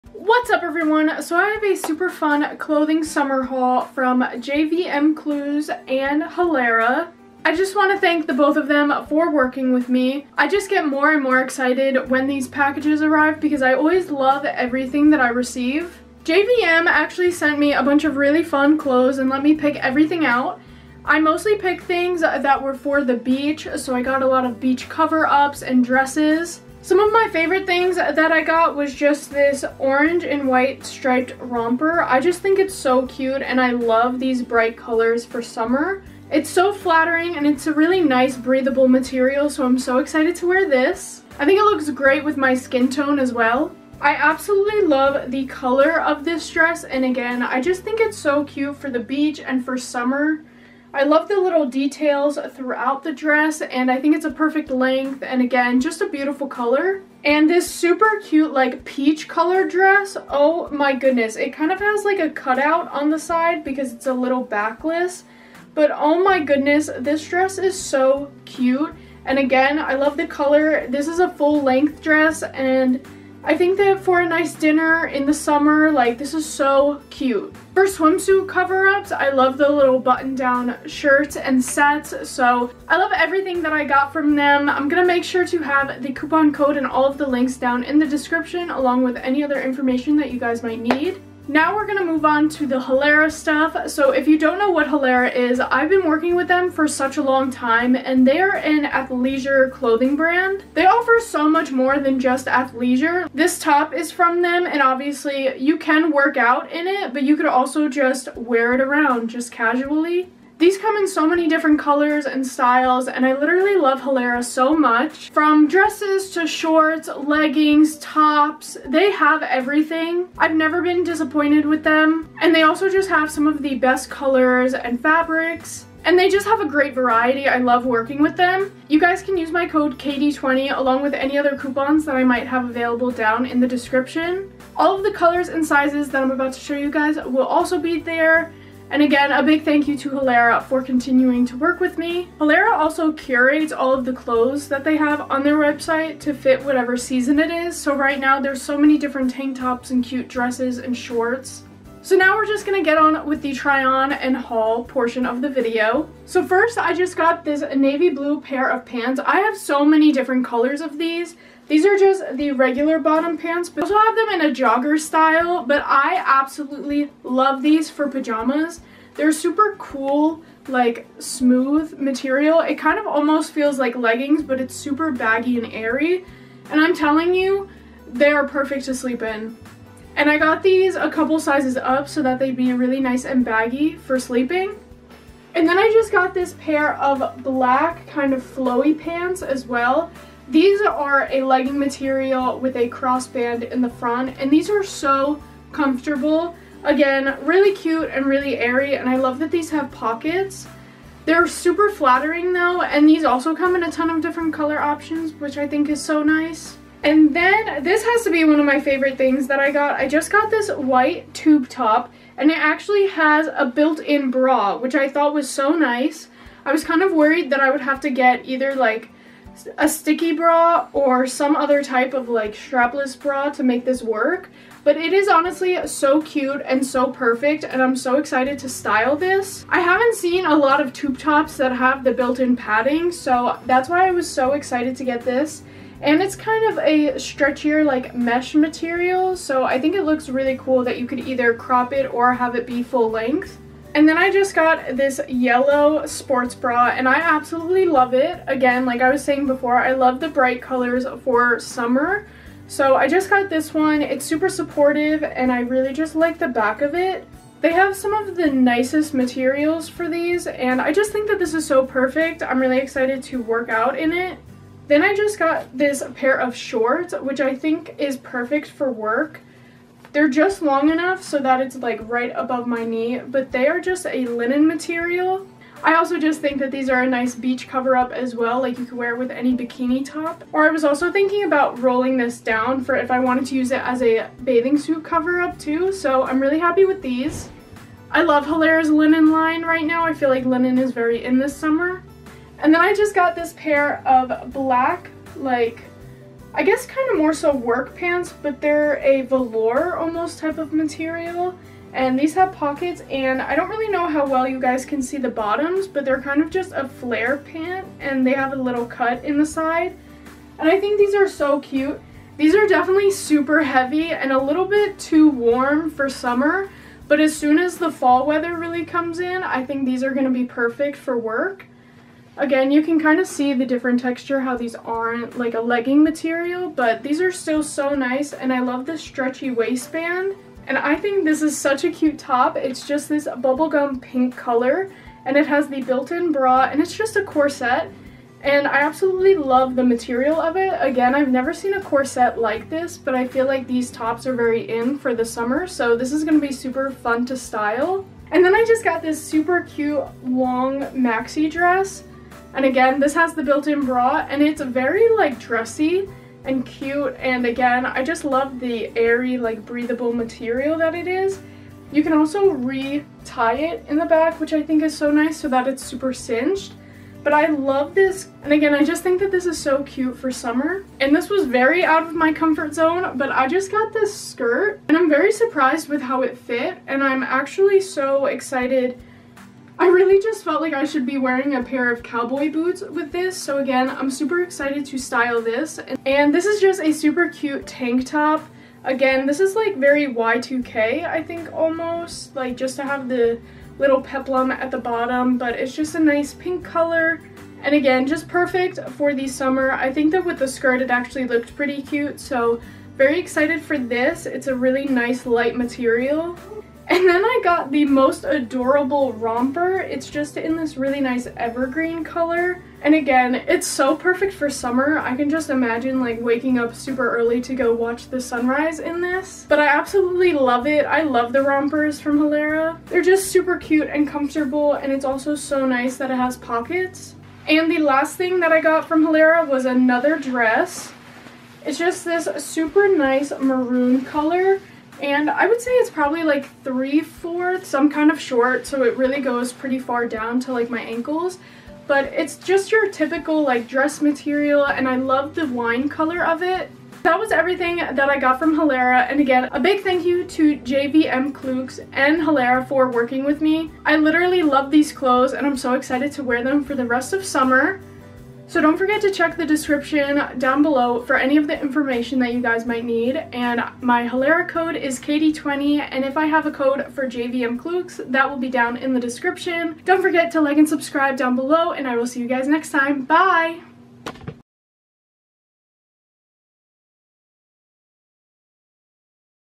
$500 Halara Haul for Summer ft JVMcluxe
Hey guys!! I have the best clothing haul yet and I'm so thankful that Halara and JVMCluxe gave me the opportunity! I got some of the best clothes for summer and I feel like everything was very affordable and cute! I love the tube top, skirt and dresses I got and I'm so excited to style everything!

Breezeful™ High Waisted Crossover Plicated Side Pocket Split Quick Dry Resort Pants
Color：Black
Size：M(regular)

Mid Rise Elastic Waistband Drawstring Side Pocket Palazzo Flowy Wide Leg Casual Pants
Color：Dark Sapphire
Size：XL

Backless Twisted U Neck Plicated Side Pocket Harem Casual Jumpsuit
Color：Dark Green Floral Yarn
Size：S

Ribbed Cowl Neck Backless Crisscross Lace Up Split Midi Party Dress
Color：Sweet Lilac
Size：M

Mid Rise Elastic Waistband Side Pocket Ruched Split Hem Corduroy Casual Wide Leg Pants
Color：Black
Size：S(regular)

HalaraMagic™ High Waisted Multiple Pockets Pleated Mini Washed Stretchy Knit Denim Casual Skirt
Color：Dark Blue Denim
Size：M(regular)

High Waisted Plicated Side Pocket Wide Leg Casual Linen-Feel Bermuda Shorts
Color：Black
Size：XS

Breezeful™ V Neck Short Sleeve Side Pocket Tie Back Quick Dry Midi Casual Dress
Color：Amaranth
Size：S
JVMCluxe:
Social Media-